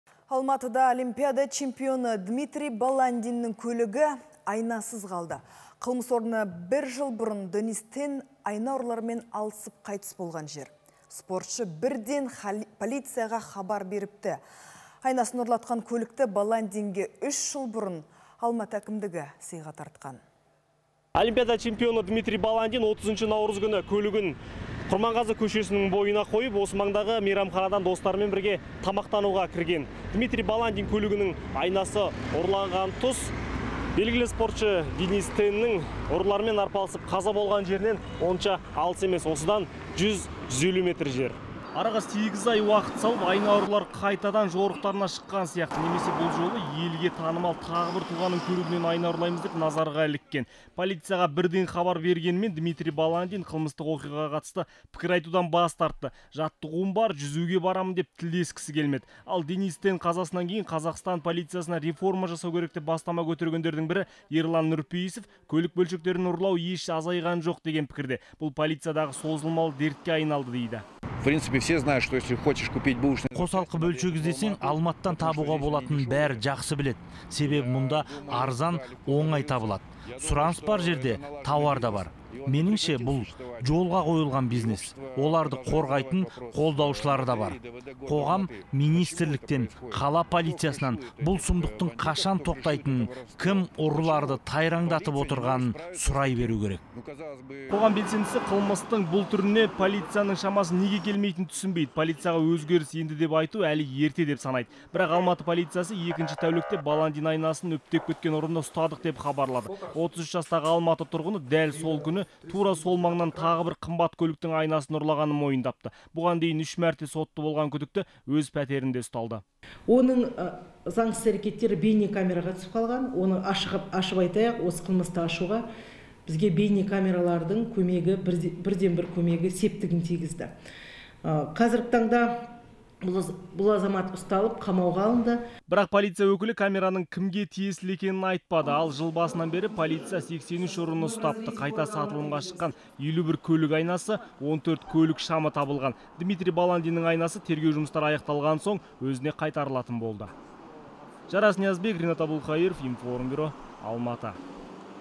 Алматыда тағы бір чемпионның көлігі айнасыз қалды
Алматыда Олимпиада чемпионы Дмитрий Баландиннің көлігі айнасыз қалды. Қылмыс орны - бір жыл бұрын Денис Тен айна ұрыларымен алысып қайтыс болған жер. Спортшы бірден полицияға хабар беріпті. Айнасын ұрлатқан көлікті Баландинге 3 жыл бұрын Алматы әкімдігі сыйға тартқан.